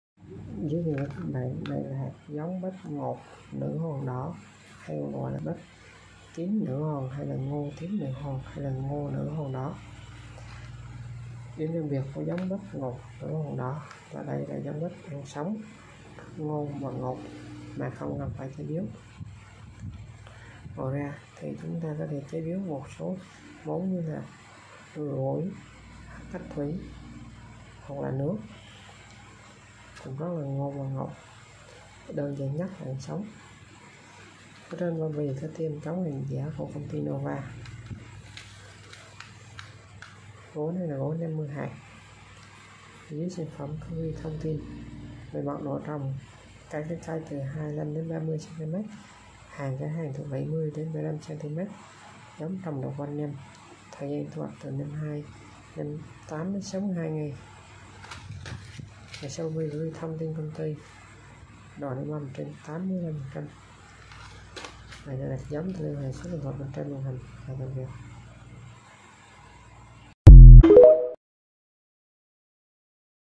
nữ hoàng đỏ Nova
Bắp nữ hoàng đỏ là giống bắp tím ăn sống rất ngon và ngọt có xuất xứ từ Thái Lan và được phân phối bởi công ty Nova.
Giống bắp nữ hoàng đỏ cho năng suất cao, trái lớn, cây sinh trưởng khoẻ, trồng được quanh năm,
Thời gian thu hoạch của giống bắp nữ hoàng đỏ từ 58 đến 62 ngày
Giống bắp nữ hoàng đỏ là giống bắp lai F1 vì thế không nên để giống cho vụ sau vì sẽ ảnh hưởng đến chất lượng giống
Không nên luộc bắp nữ hoàng đỏ vì sẽ mất đi vị ngọt cũng như màu sắc đặc trưng của bắp nữ hoàng đỏ
Bắp nữ hoàng đỏ ngon nhất là ăn sống, ngoài ra có thể chế biến bắp nữ hoàng đỏ thành nhiều món như gỏi, salad, chè trân châu, bắp nữ hoàng đỏ hấp cách thủy.
Quy cách đóng gói: 50 hạt